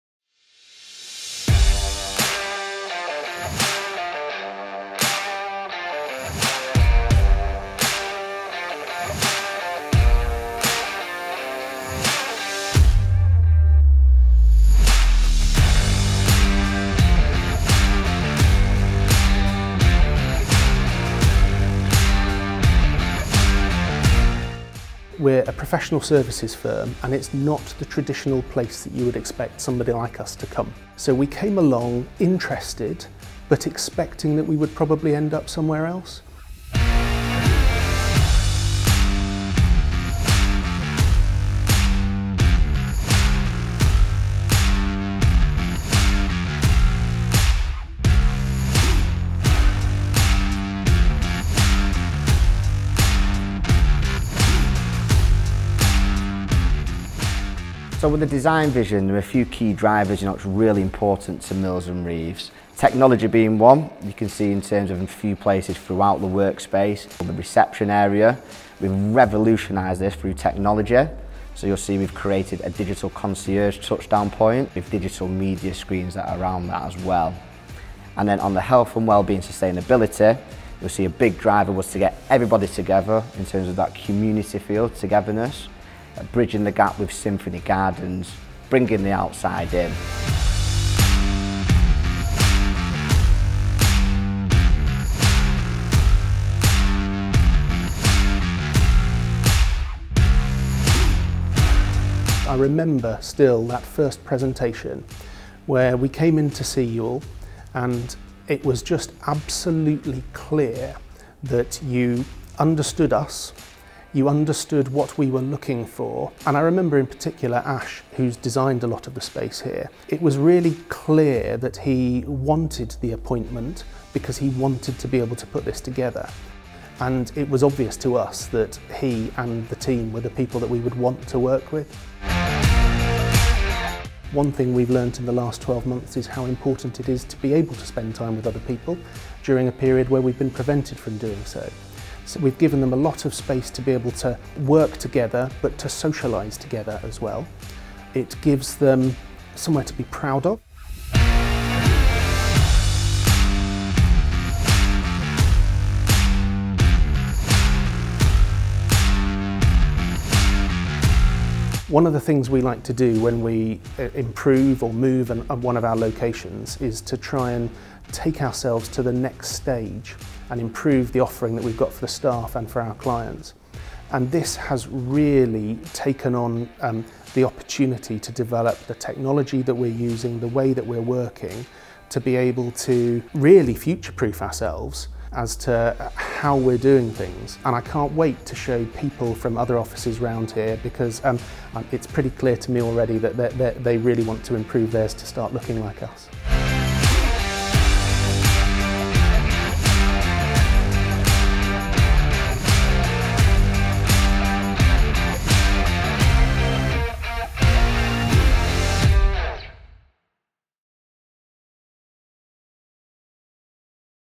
Mills and Reeve Office Fit Out - Circle Square, Manchester
Step inside this Cat B fit out of the 5th floor of 1 Circle Square, for legal company Mills & Reeve.

Mills & Reeve's driver to relocate to Circle Square, Manchester was not only to become the lawyer of choice for the tech and creative industries, but to bring their staff together on one floor plate whilst implementing new ways of working.

The vision was to create an environment that enhances the Mills & Reeve brand, invites innovation, agility and collaboration - providing a destination that clients, visitors and colleagues will choose to go.